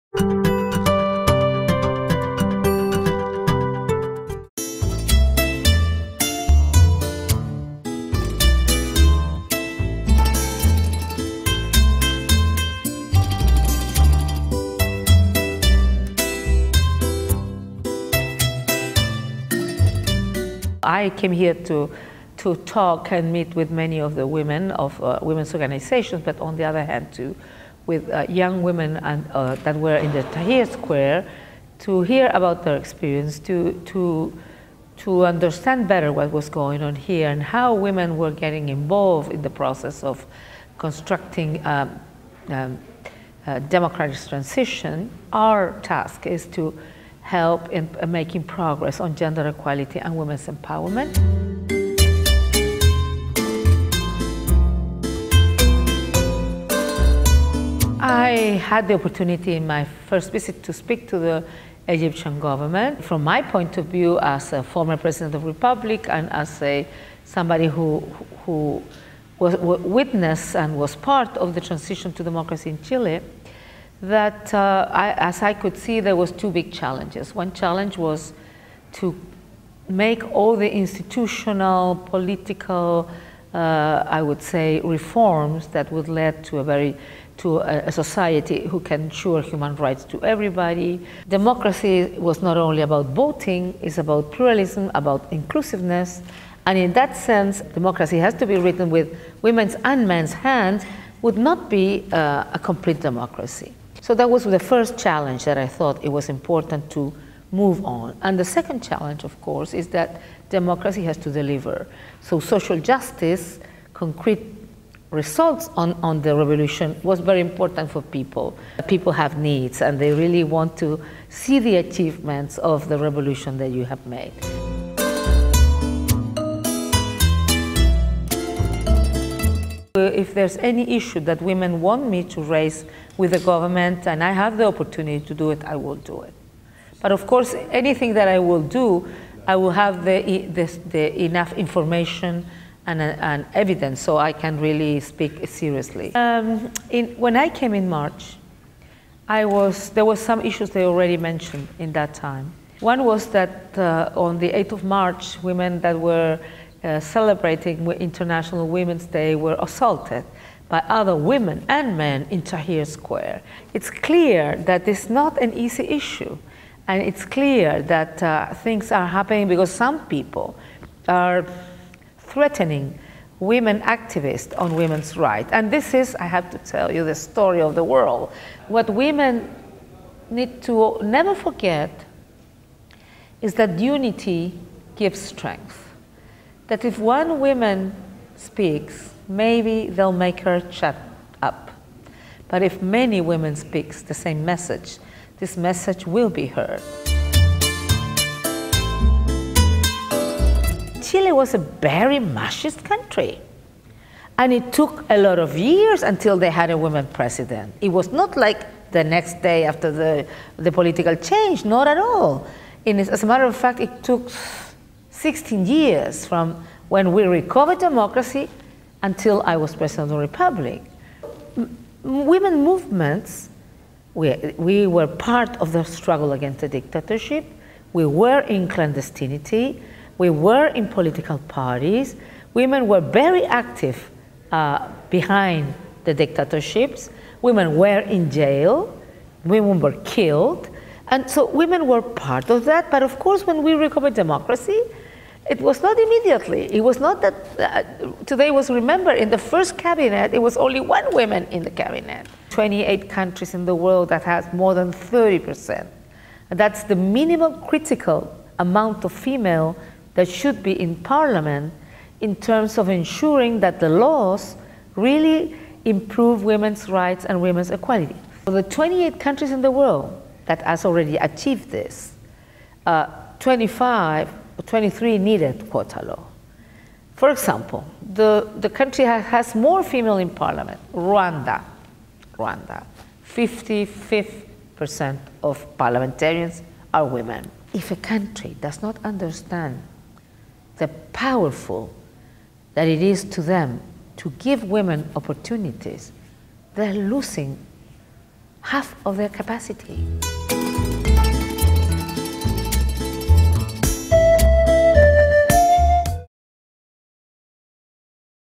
«الأمم المتحدة» تدعو المصريات لكتابة «الدستور»
أكدت المديرة التنفيذية لهيئة الأمم المتحدة لشؤون المرأة، ورئيسة جمهورية تشيلي السابقة، ميشيل باشيلي على ضرورة مشاركة المرأة المصرية في كتابة الدستور، وقالت: « ديمقراطية لا تكتب بأيدي النساء لا يمكن أن تكون ديمقراطية كاملة». أشارت باشيلي إلى تجربة تشيلي في الإصلاح الديمقراطي، وقالت إن المرأة احتاجت 16 عاما لتصل إلى كرسي الرئاسة.